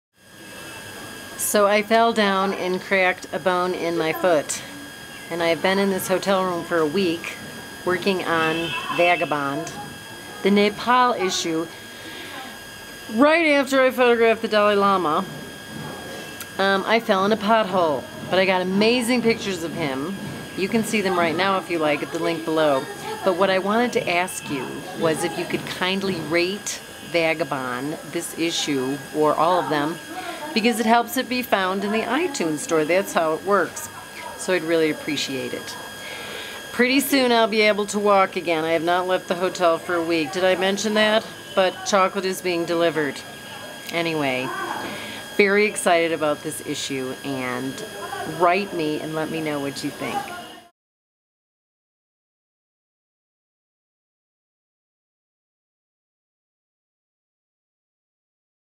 Vagabond Mag and Photographs of the Dalai Lama
Yes I cracked a bone in my foot (at least it wasn't my head) and am waiting for the bones to fuse together. Meanwhile I am using a mop handle for a cane but trying to obey the doctor's orders and stay put on my bed. If you want to see Dalai Lama Photos I shot last week go
Starting at $15 prints delivered to your door in the USA and Europe. Also Downloads.